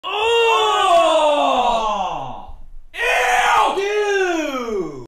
My Reaction To Skibidi Toilet Gettting A Movie And A Series
JESUS CHRIST WHAT THE HECK IS THIS?! THERE IS NO WAY I’M WATCHING THAT THIS IS SO HORRIBLE!👎👎👎😡😡😡🤦‍♂️🤦‍♂️🤦‍♂️